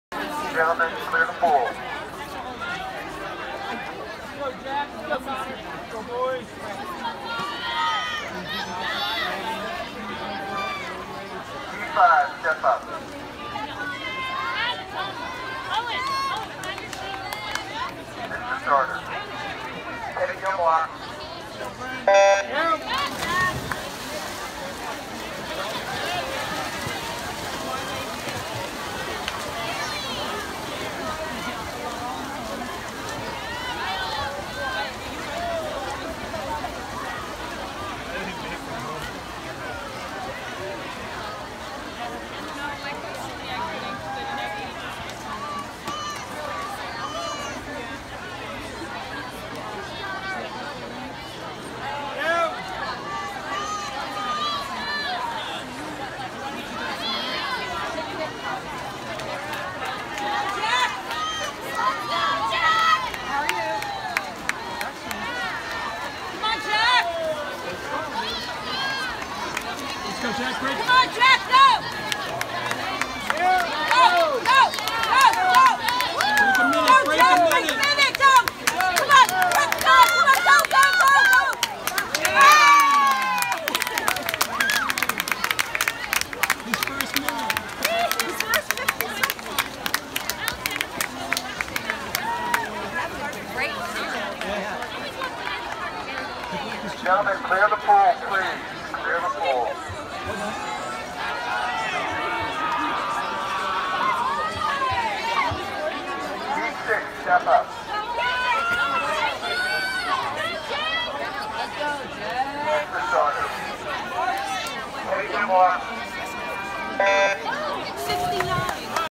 2018 Westchester County Championships
Day 1: Boys 14U 100 Free
Jack Breaks 1:00:00 Mark
59:45